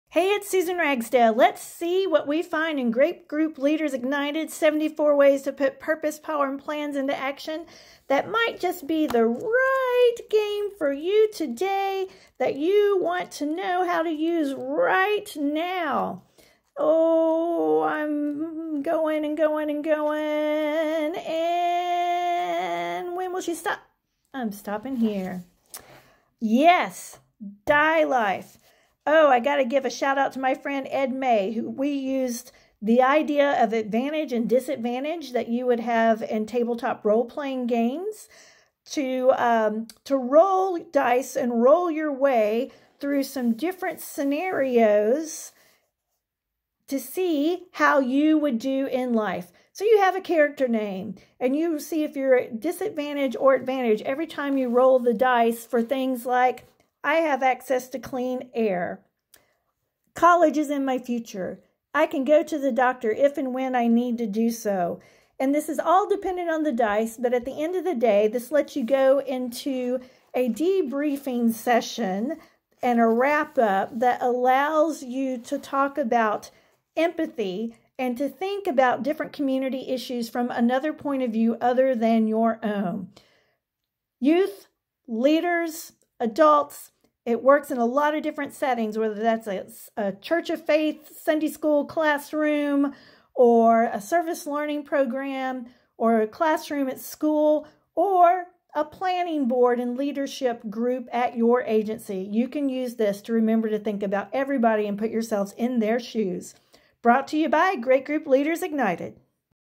Die Life - A Game to Encourage Empathy in Leaders
Rolling dice. Taking on characters of the community. Seeing if you succeed or fail in meeting life's challenges.

Die Life, from Great Group Leaders Ignited! 74 Ways to put Purpose, Power, & Plans Into Action, lets groups take on another's persona and see what it's like to be them as life throws challenges their way. The debriefing of this activity lets leaders, young and old, consider impact on others, promoting empathy, thoughtful reflection, and critical thinking.

Links to order this book:

Print version

Want more ideas for engaging your group?